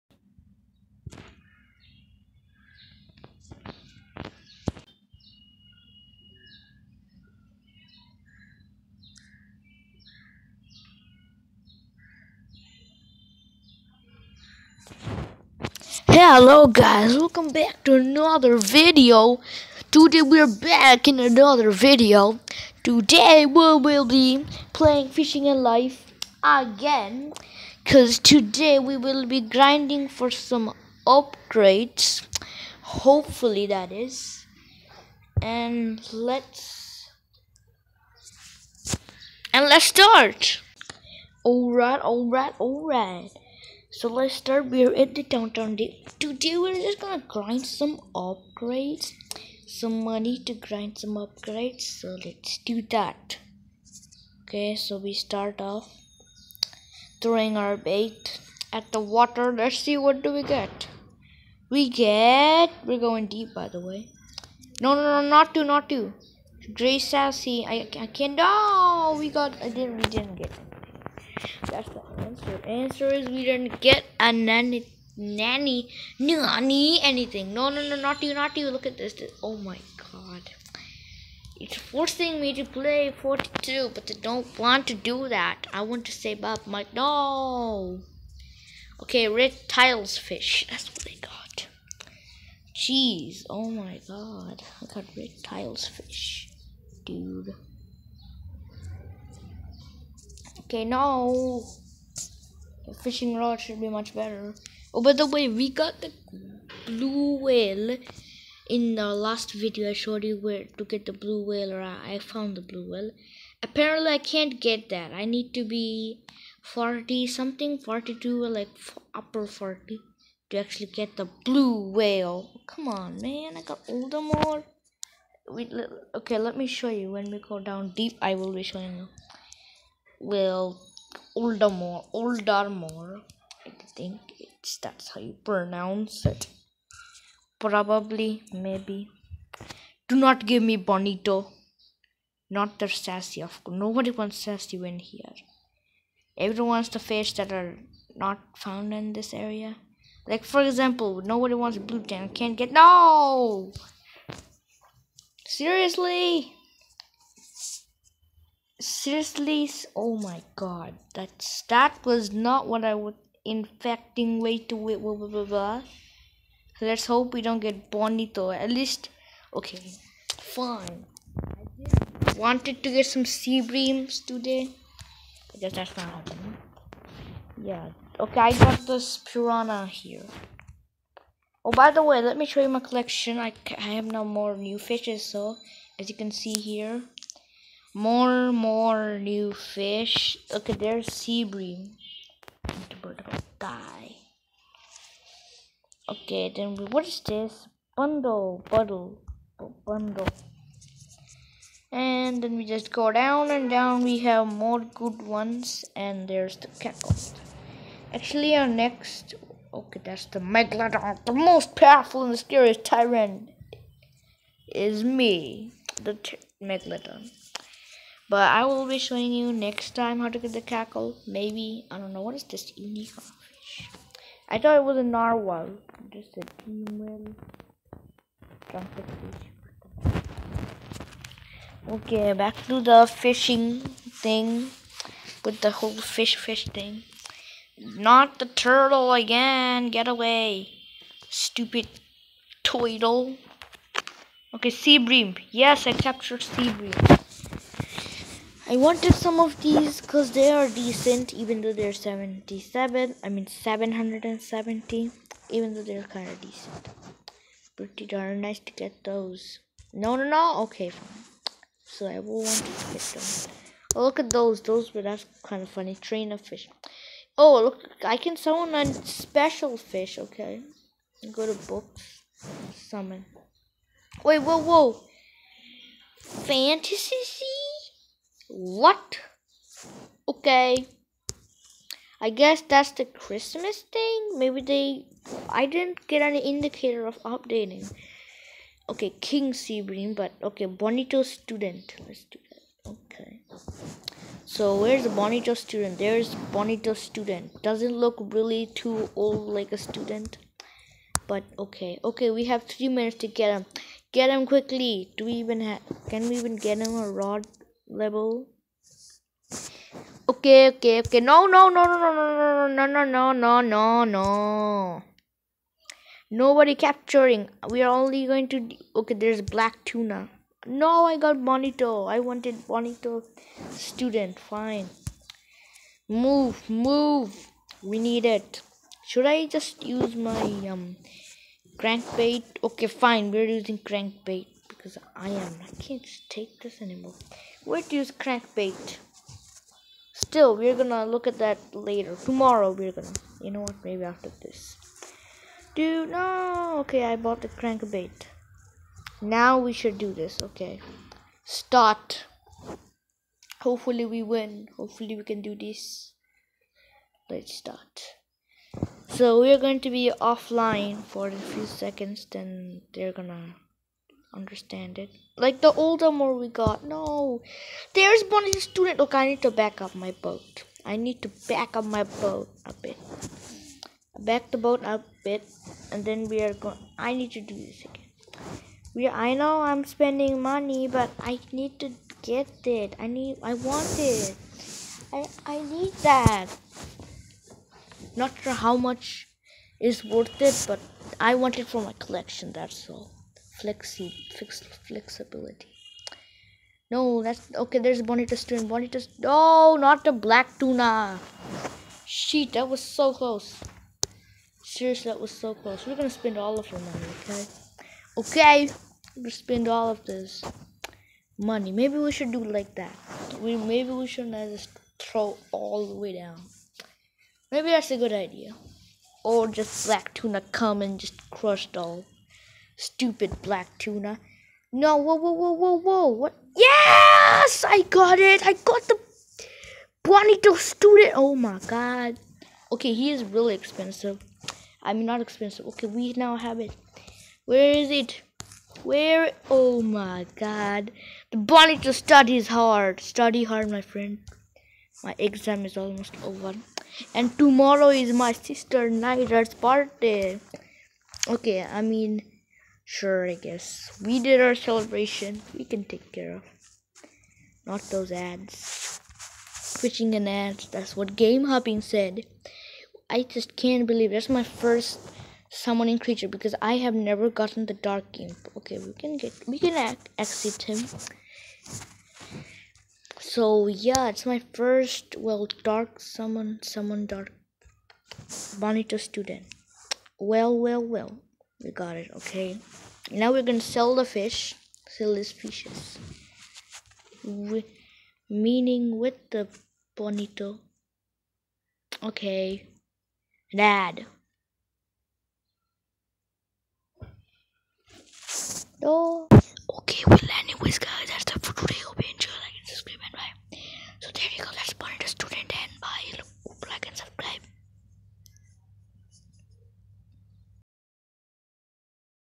Fishing & life. I GOT BONITO STUDENT!!
Skip to 7:00 if u want to see me getting bonito student.